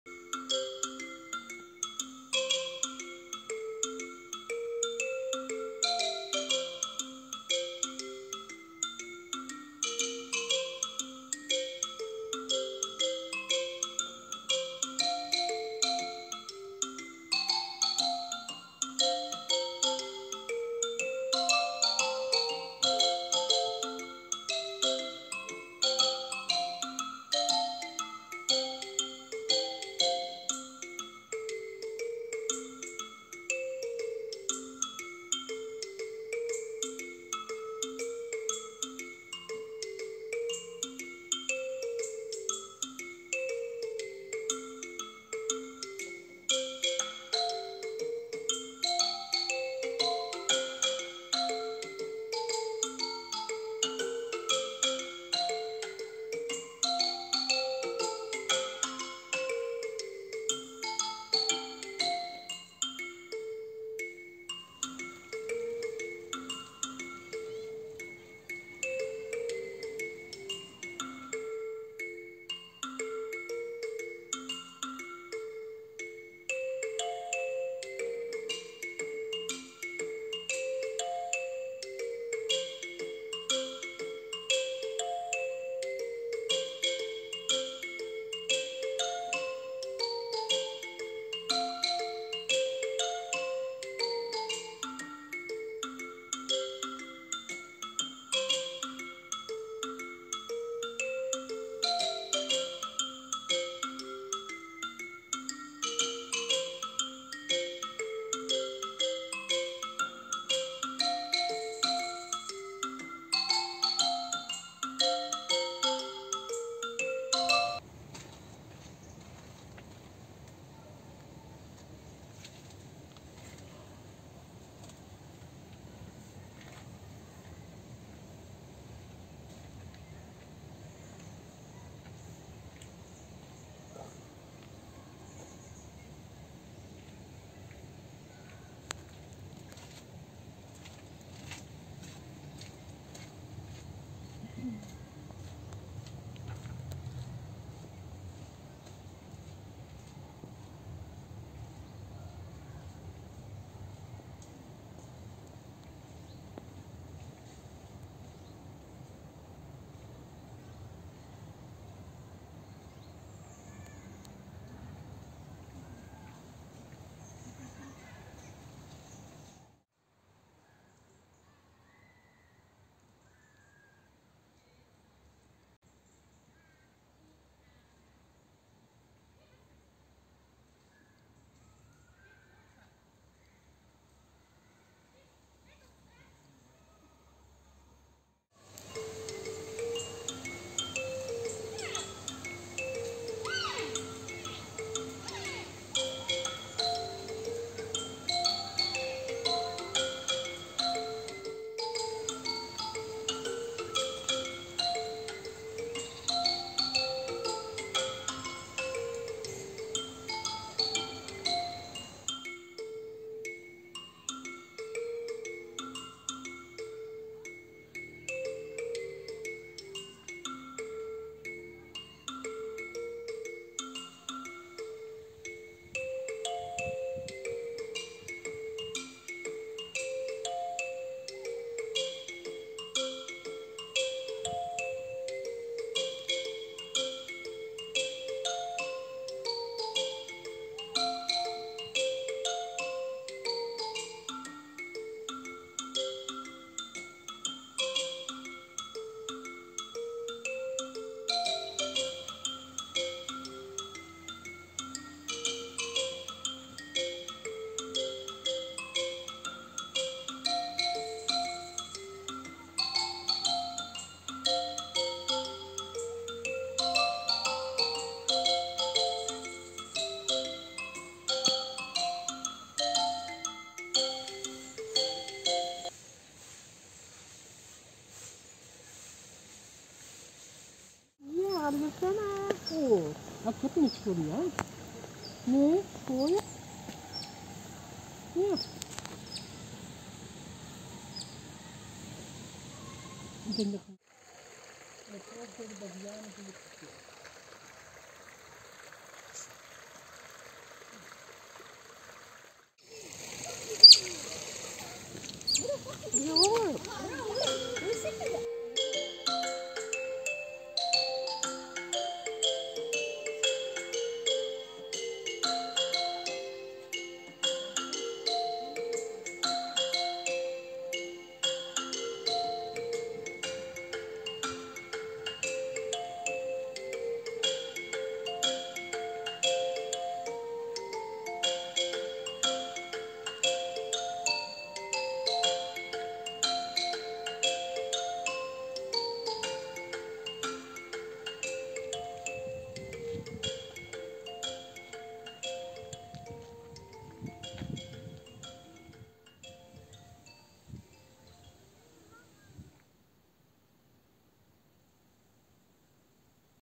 4 October 2023 - WORLD ANIMAL DAY - Zoo Planckendael (B) - zoovenirs (53)
During my visit of Zoo Planckendael on 22/09/23 , l wanted to get animal species on camera that l hadn't been able to show yet. And l managed to do so, even beyond my expectations.
As several of these animal species are on the endangered species list, l thought of uploading the video today.
4 October is World Animal Day. To celebrate this day, it is the custom to give extra attention to our pet friends, but also to consider animal rights and animal protection. Not only for domesticated animals, but also for animals in the wild.
So why on 4 October? It started in 1930, as a group of animal protectors chose this day as World Animal Day, because it's the day Fransiscus of Assisi, patron saint of animals, died in 1226.

l don't claim any rights.